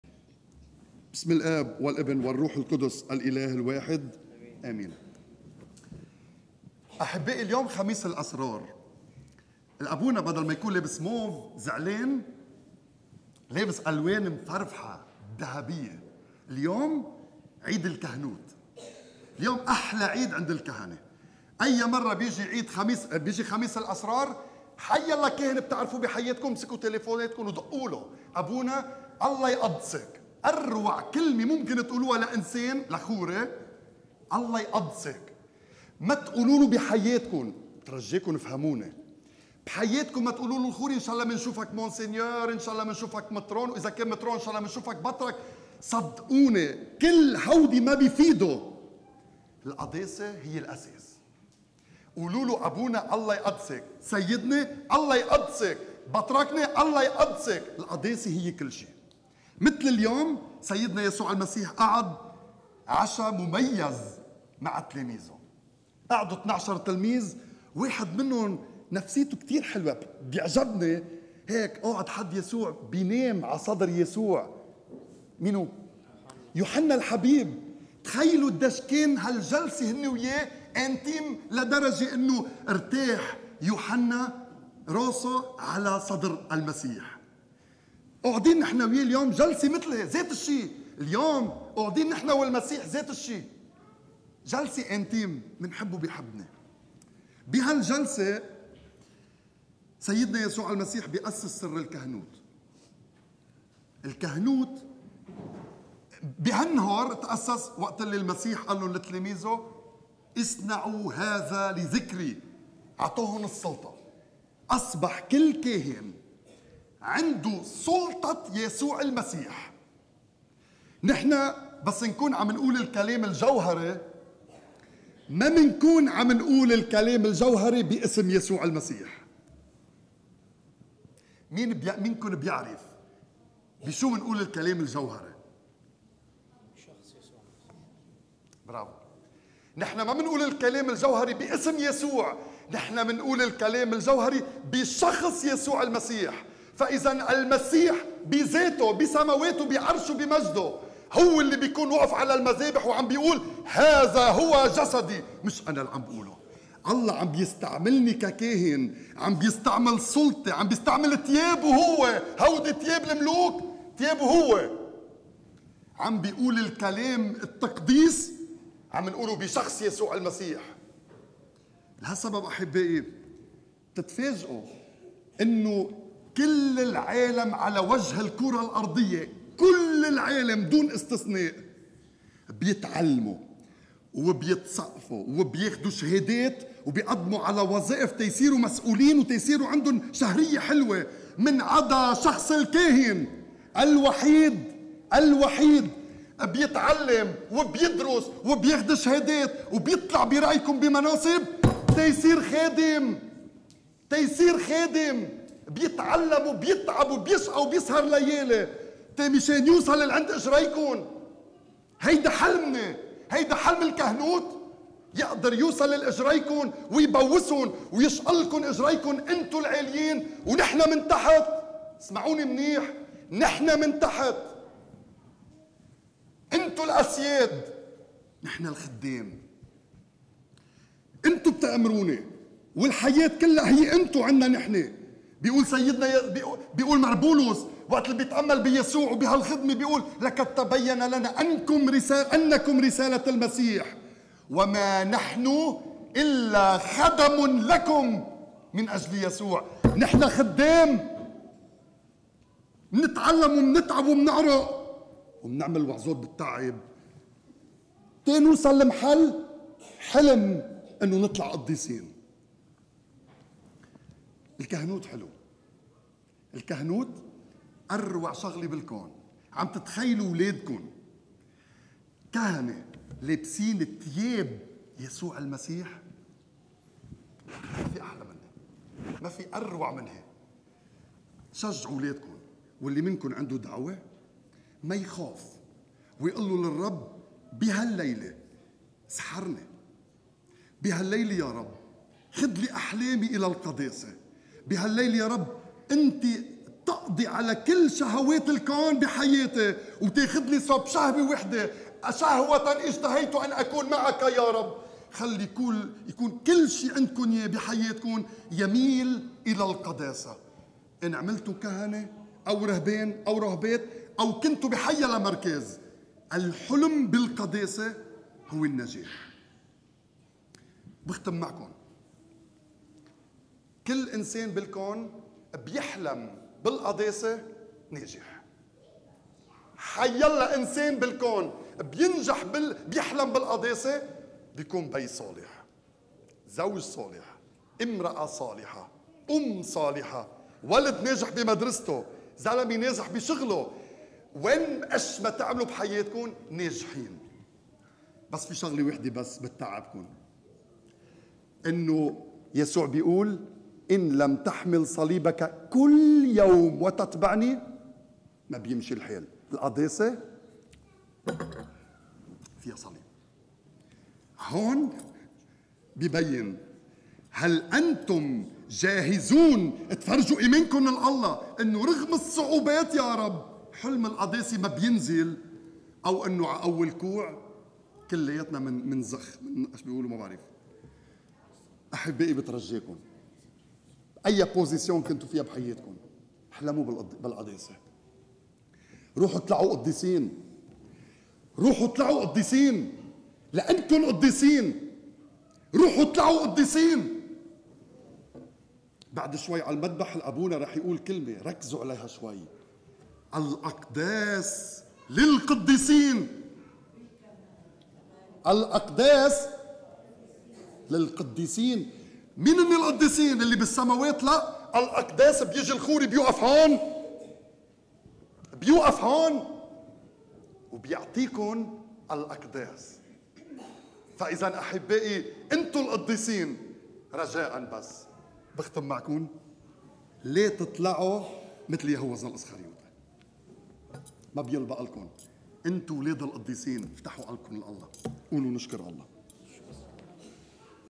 عندك دعوة كهنوتية ومحتار؟ إسمع قبل اتخاذ القرار
يوم خميس الأسرار أسس إلهنا يسوع المسيح سر الكهنوت. ولكن كيف يمكن لنا أن نعيش حياتنا الكهنوتية؟ ما هو الحلم الأول والأخير لنجاح الخدمة الكهنوتية؟

خميس الأسرار. كنيسة مار شربل، بنسلفانيا
الخوري جوزيف الضعيف
نيسان 2023